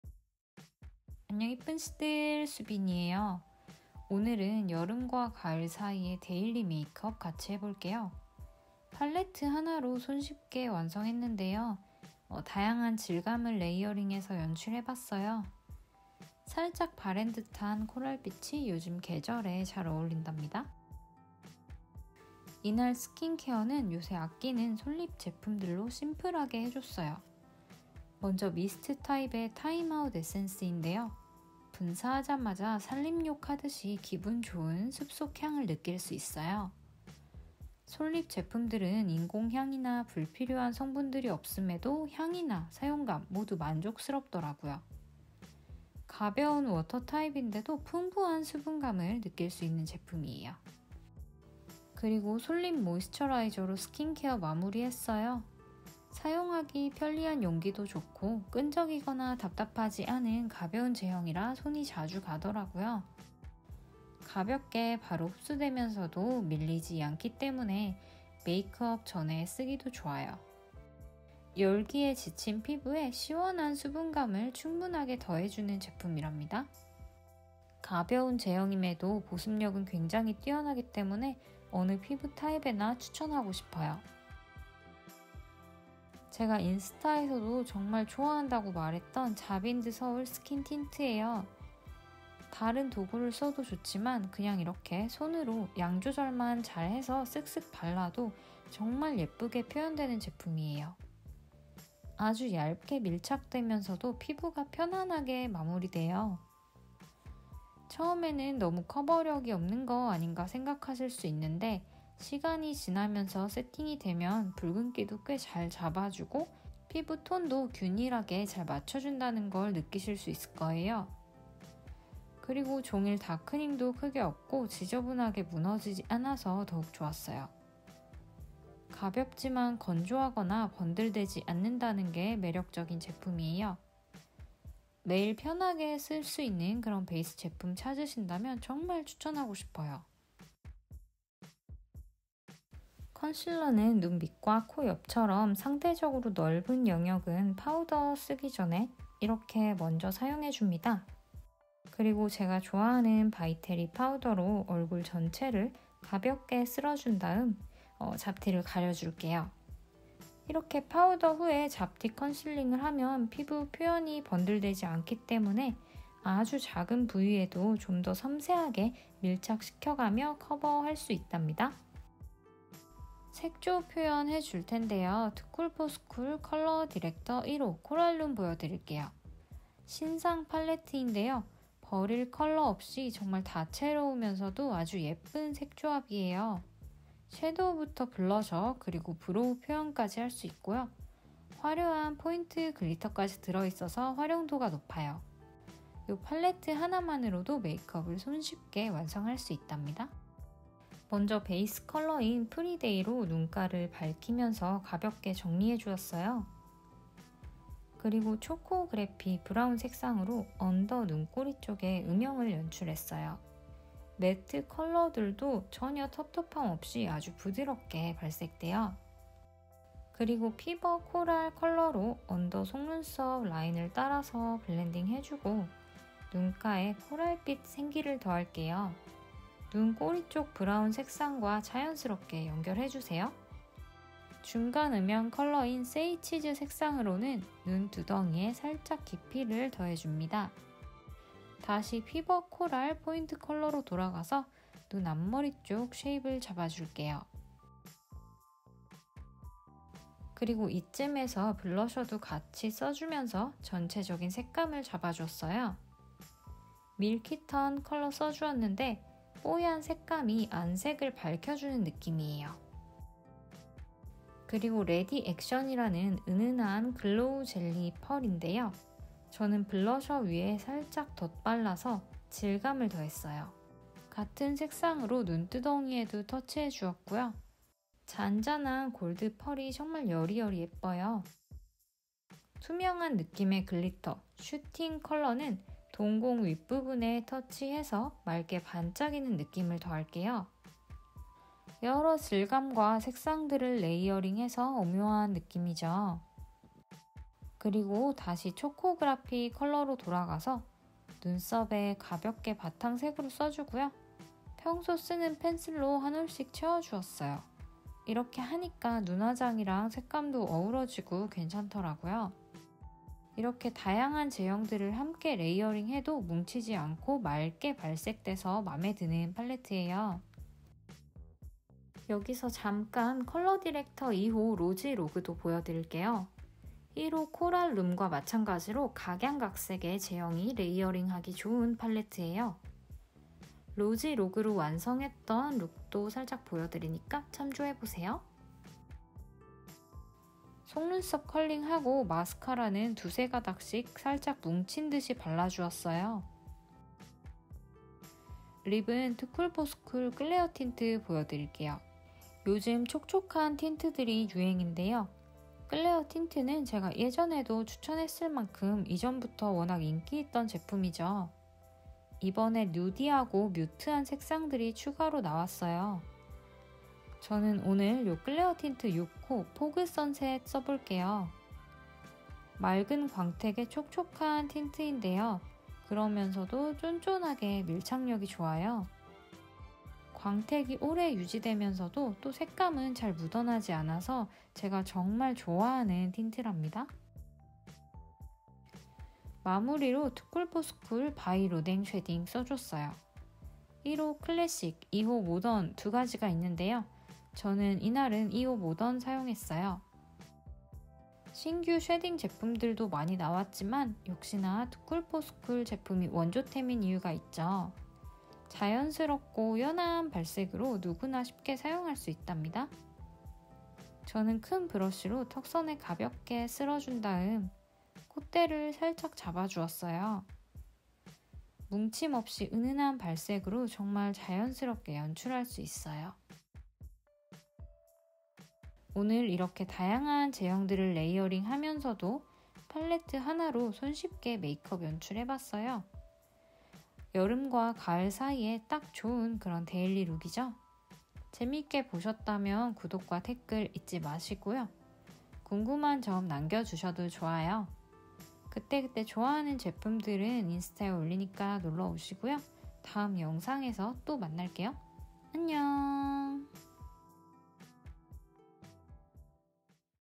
가을일까? 뮤트코랄 데일리 메이크업 | 투쿨포스쿨 신상들로 GRWM🧡 DAILY CORAL MAKEUP with TOO COOL FOR SCHOOL
컬러디렉터 1호 코랄룸 / 2호 로지로그

끌레어틴트 6호 포그 선셋 / 7호 로지스웨이

투쿨포스쿨 바이로댕쉐딩 1호 클래식 / 2호 모던

🧡사용한 제품들

SOLYPH Time Out Refresh Forest Bathing Essence Mist
솔잎 타임아웃 리프레시 포레스트 베딩 에센스 미스트

SOLYPH Ready, Set, Glow Multi-Action Moisturizer
솔잎 레디 셋 글로우 멀티 액션 모이스쳐라이저

jennyhouse Silk Fit Liquid Concealer, 02
제니하우스 실크 핏 리퀴드 컨실러, 02

Mustaev  Kowonhye Concealer Brush 08
머스테브 고원혜 컨실러 브러쉬 08

By Terry Hyaluronic Hydra Powder
바이테리 히알루로닉 하이드라 파우더

Piccasso 133 Fixing Brush
피카소 133 픽싱 브러쉬

Piccasso 505 Brush
피카소 505 브러쉬

TOO COOL FOR SCHOOL Artclass Color Director, 1 Coral Room
투쿨포스쿨 아트클래스 컬러디렉터, 1호 코랄룸

Piccasso  207A Brush
피카소 207A 브러쉬

Piccasso 777 Brush
피카소 777 브러쉬

ZOEVA 228 Luxe Crease Brush
조에바 228 럭스 크리즈 브러쉬

ZOEVA 322 Brow Line Brush
조에바 322 브로우 라인 브러쉬

Filli Milli Heated Eyelash Curler
필리밀리 히팅 뷰러

peripera Ink Black Cara, Clear-set Curling
페리페라 잉크 블랙 카라, 깔끔 컬링

TOO COOL FOR SCHOOL Color Director, 2 Rosy Log
투쿨포스쿨 컬러디렉터, 2호 로지로그

TOO COOL FOR SCHOOL By Rodin Shading, 1 Classic, 2 Modern

Ohgnus Shadow & Shading Brush 013
오그너스 쉐도우&쉐이딩 브러시 013

Piccasso 201 Brush
피카소 201 브러쉬

🧡FIND ME